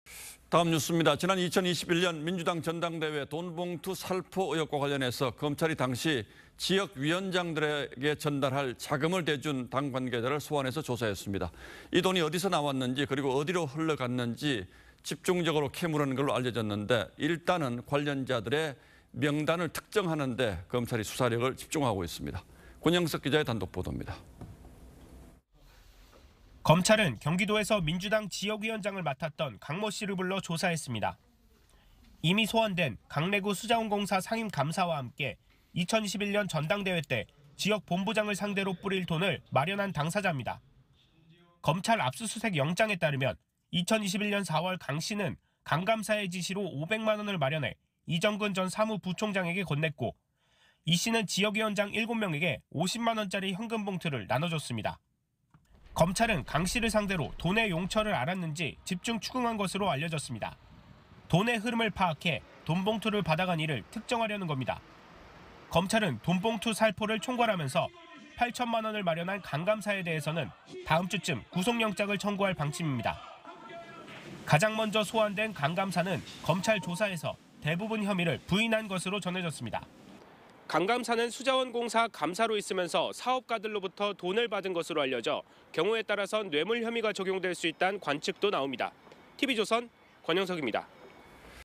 [단독] 檢, '지역본부장 돈봉투' 조성자 소환…흐름 추적
2021년 민주당 전당대회 돈봉투 살포 의혹과 관련해 검찰이 당시 지역위원장에게 전달할 자금을 대 준 당관계자 를 소환 조사했습니다. 이 돈이 어디서 나왔는지 그리고 어디로 흘러갔는지를 집중적으로 캐 물은 걸로 알려졌습니다.